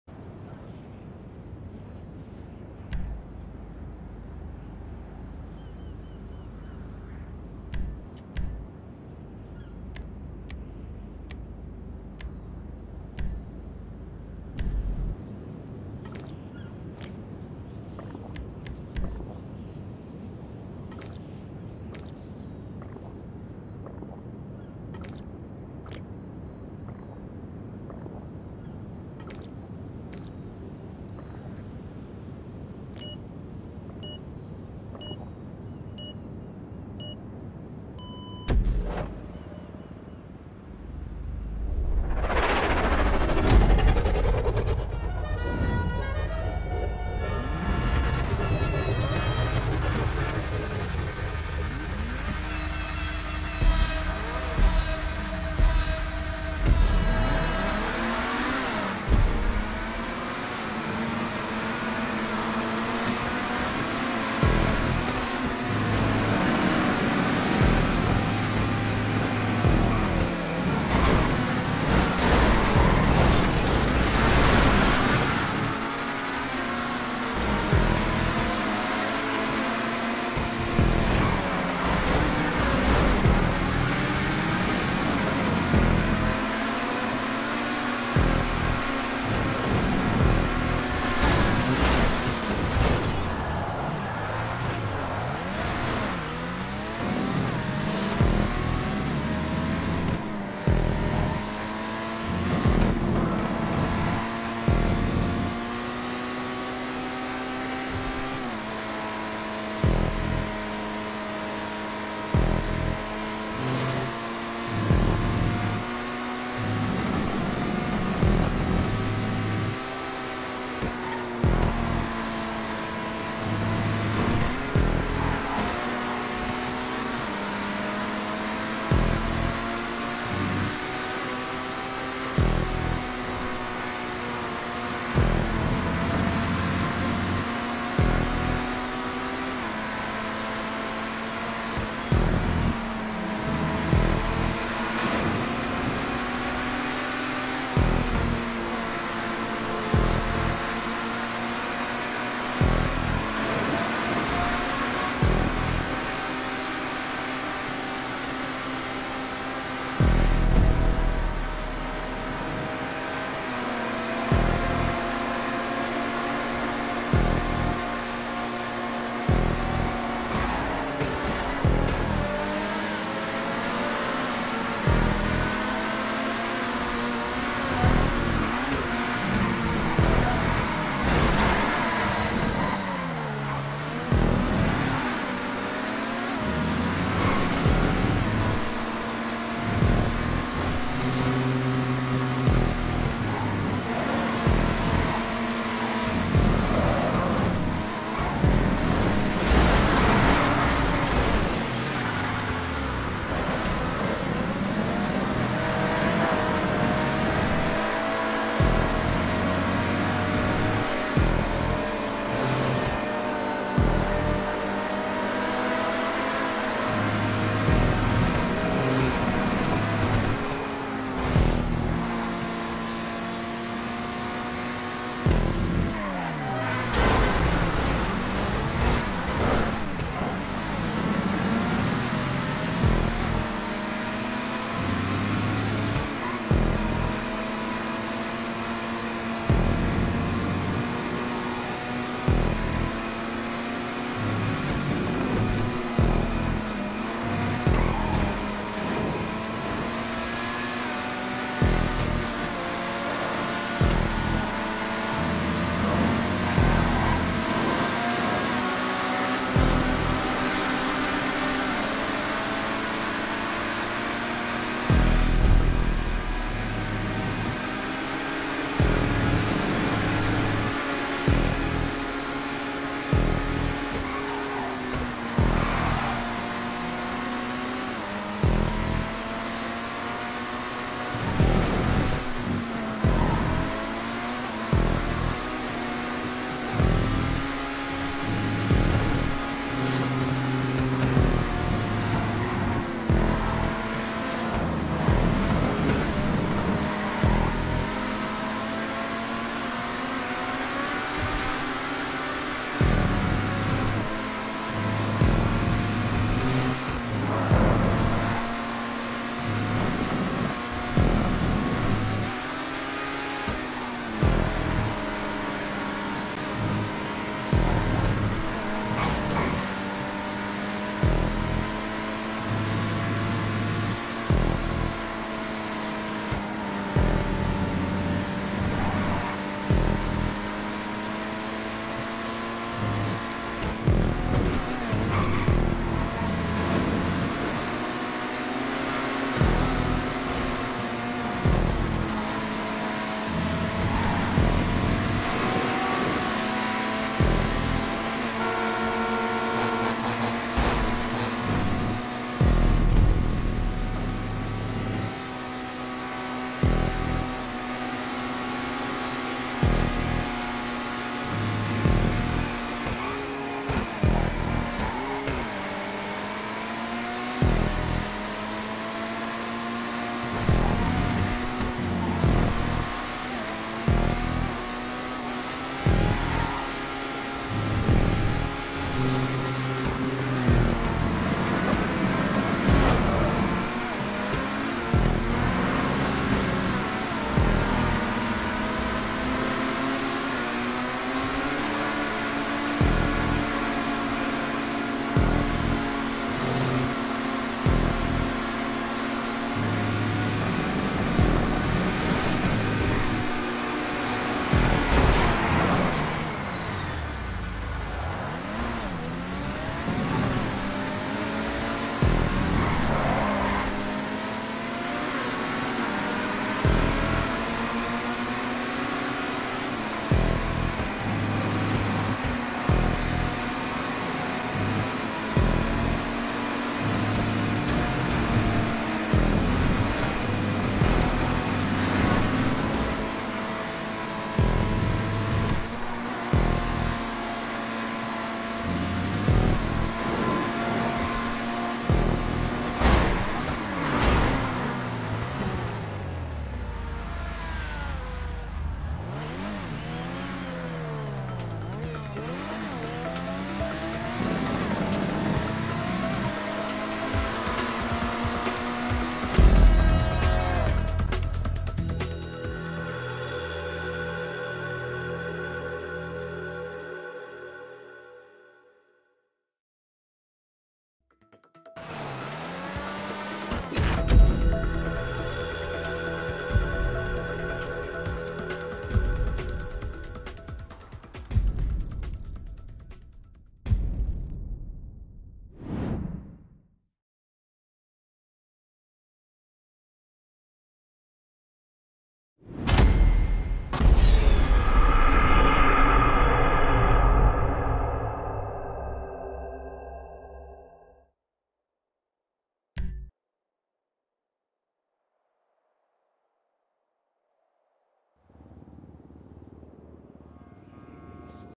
Little Pollo the Jerk
During Circle Sunday pl, in a Supers race, Pollo takes out the leader, others and me at the end. Showing the entire race from my view. He does this all the time.